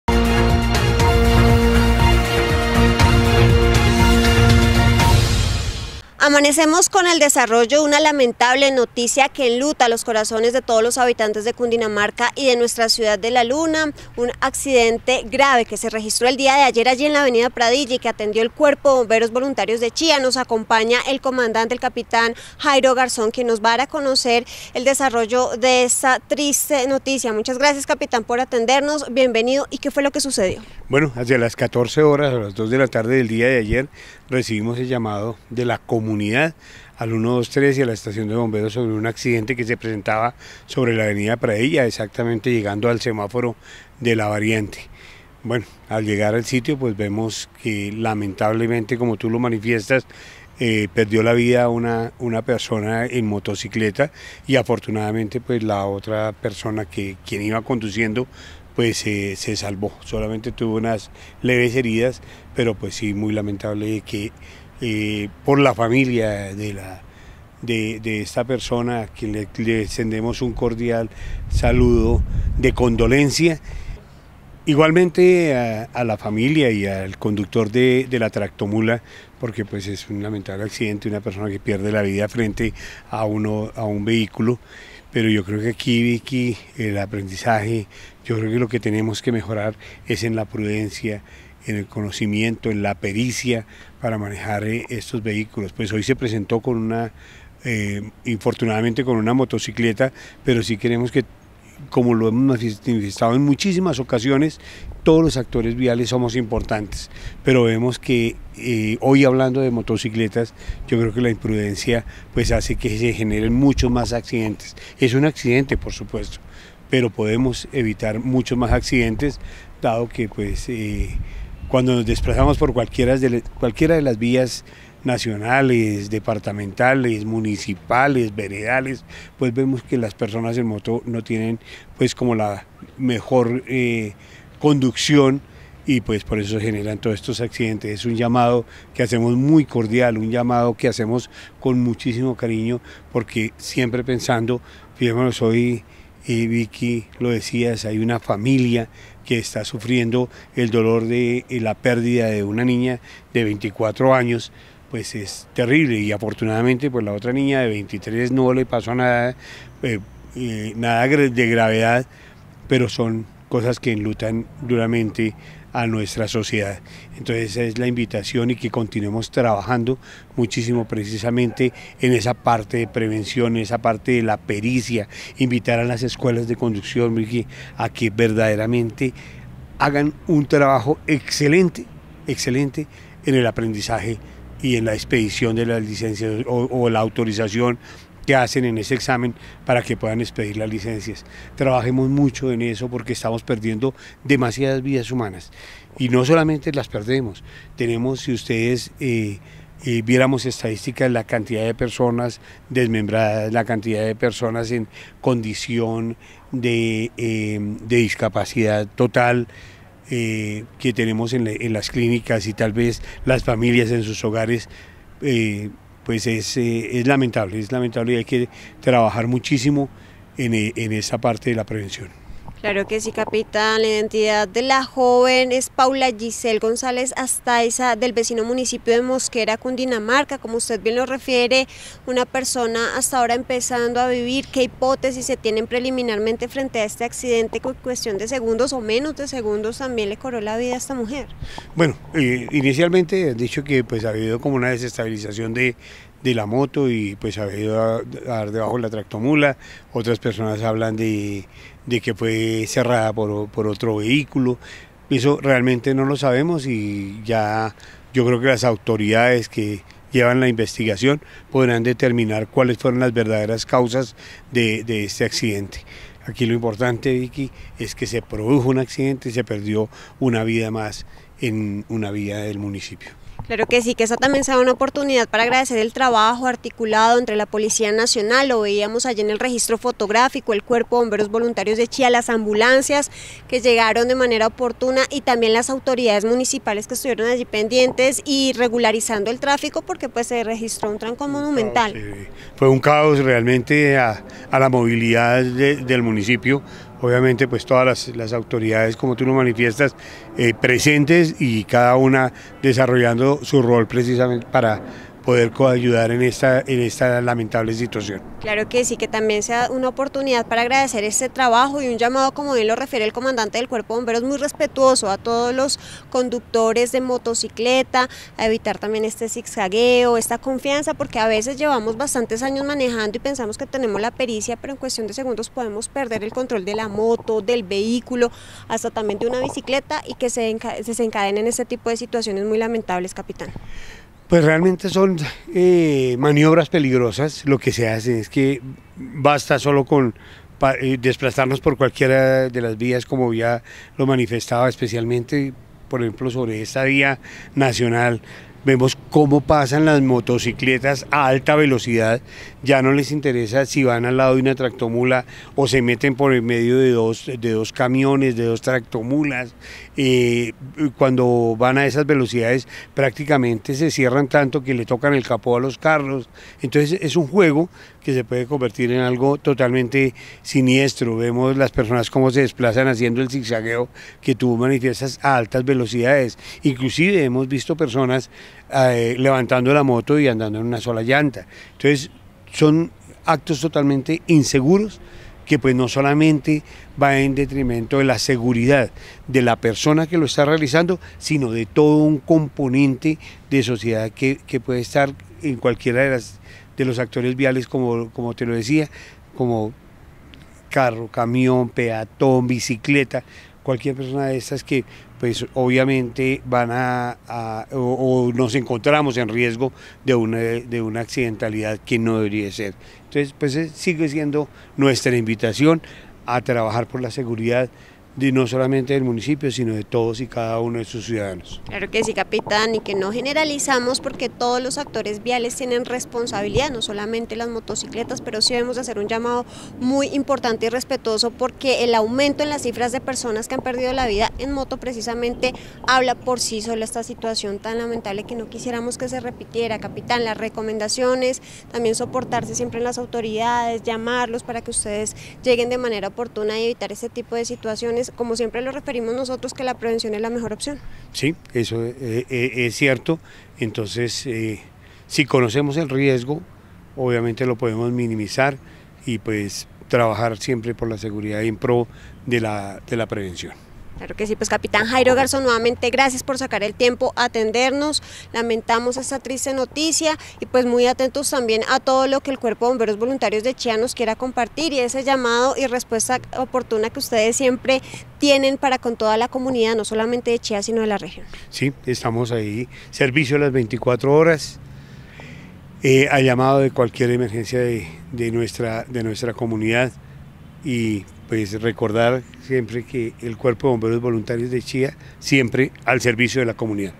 Capitán de Bomberos de Chía habló sobre el lamentable accidente donde una joven perdió la vida.
El comandante del Cuerpo de Bomberos de la ciudad entregó los datos de la joven que infortunadamente perdió la vida en el accidente de tránsito registrado en la Av. Pradilla de Chía, se trata de Paula Guissel González Astaiza de 23 años de edad, su acompañante fue identificada como Karen Gicela Duarte Melo de 25 años, quien se encuentra en un centro asistencial con trauma de tejidos blandos, ambas jóvenes residentes del municipio de Mosquera Cundinamarca, la placa de la moto y las características; Honda KKC- 62E.

Lamentamos profundamente la perdida y enviamos un saludo fraterno de condolencias a su familiares y amigos, paz en su tumba.